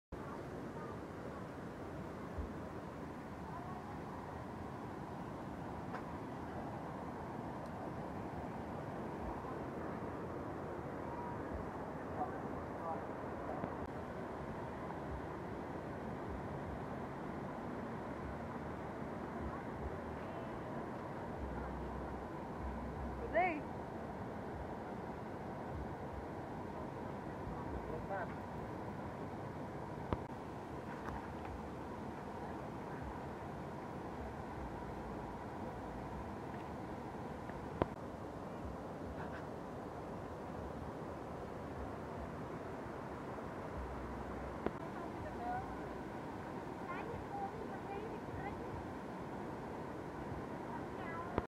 នៅភូមិអភិវឌ្ឍន៍ អន្លង់ជ្រៃ យើងហ្នឹង / Chhan Zeki
Here’s the water flow that the people come to play and swim. It’s located in Onlung Chrey village in Kompong Cham province.